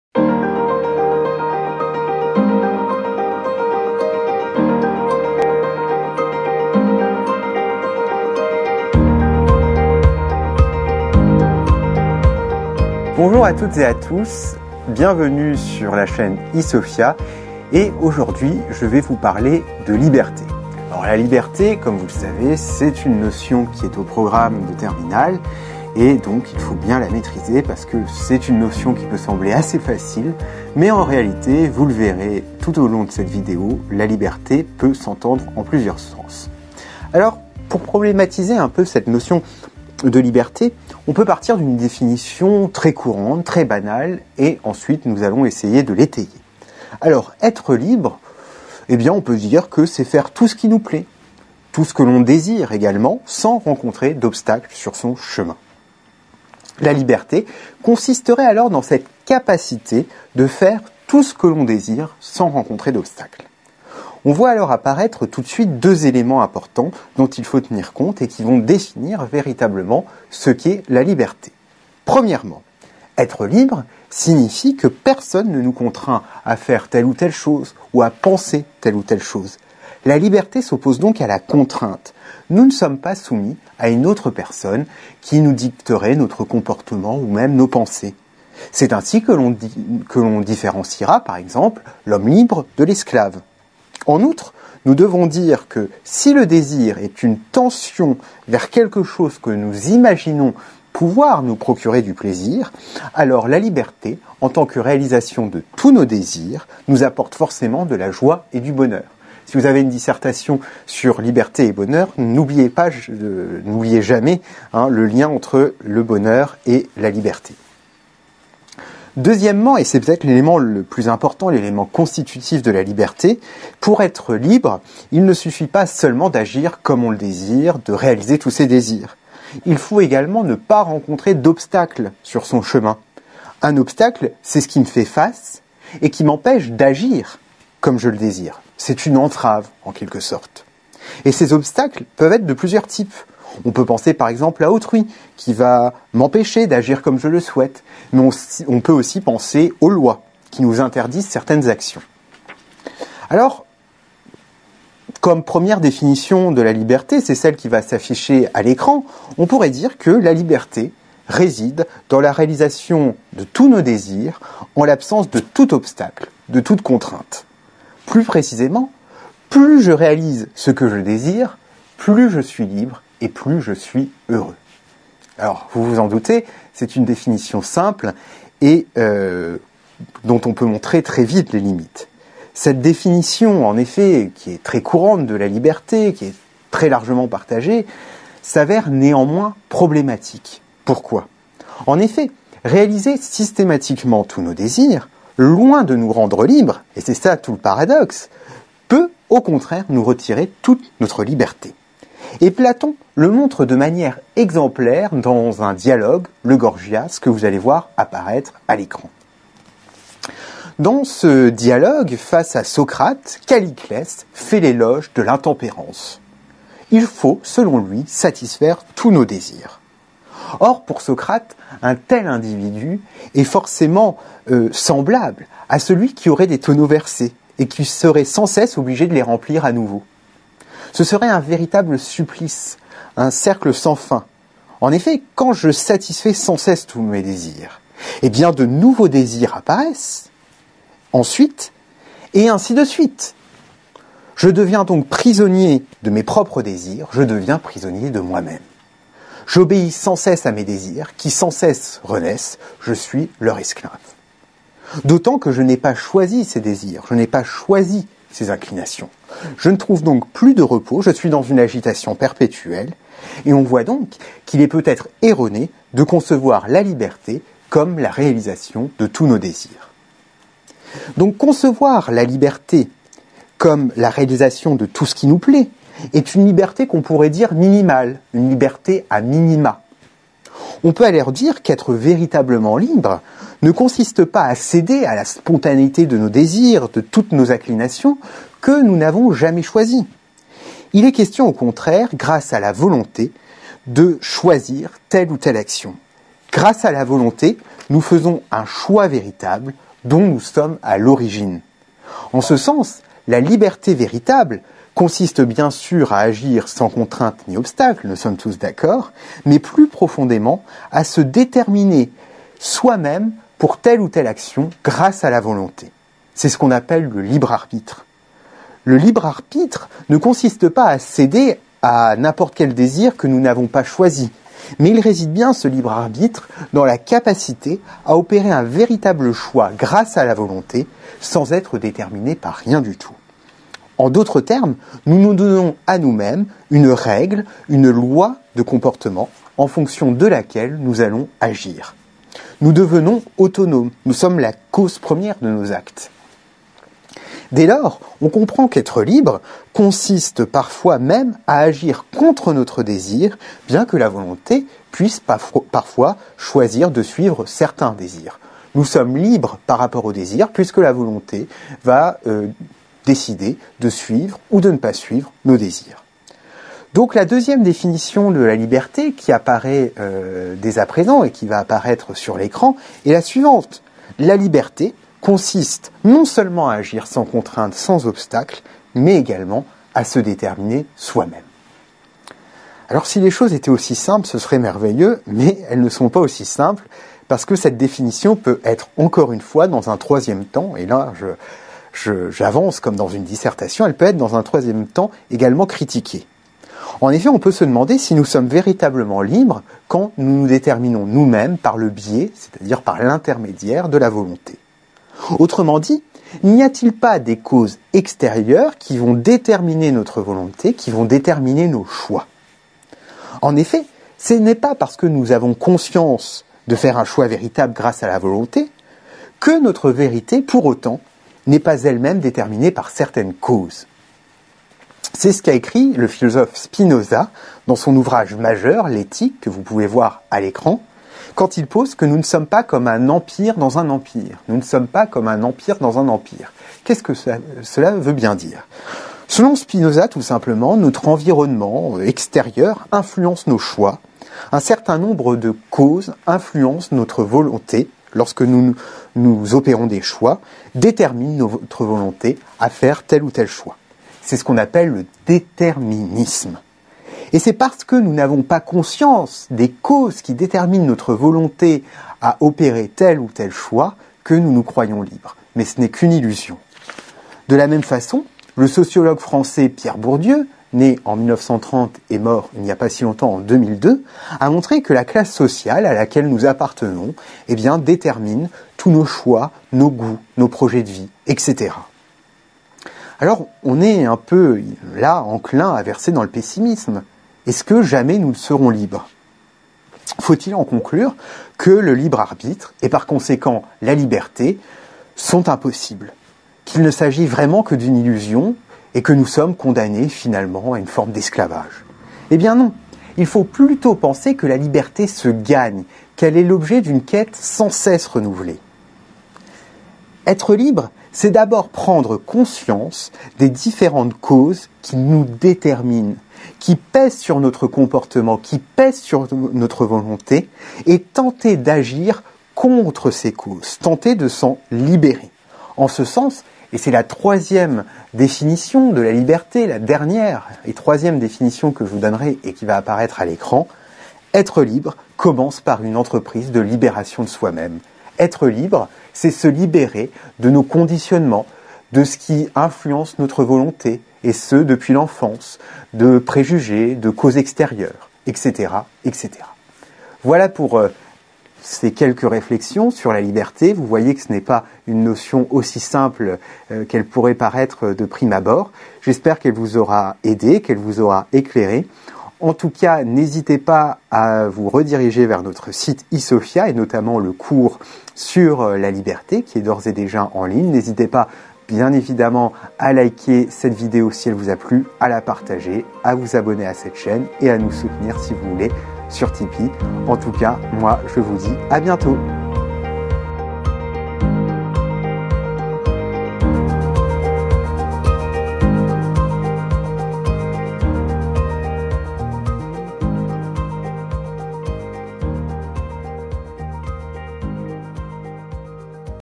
Qu'est-ce qu'être libre ? Les indispensables du bac philo #8
On définit souvent la liberté comme cette capacité de faire tout ce que l'on veut sans rencontrer aucune contrainte. Toutefois, en agissant ainsi, on risque de devenir l'esclave de soi-même et de ses propres désirs. De fait, la liberté implique une certaine maîtrise de soi qui passe par une libération de soi-même, de tout ce qui nous détermine malgré nous. En ce sens, être libre c'est se déterminer soi-même, rationnellement.

⚠️ Edit: Il y a une erreur d'intégration à 7'13''. C'est la couverture du livre L' Éthique de Spinoza qui aurait dû s'afficher. Toutes nos excuses !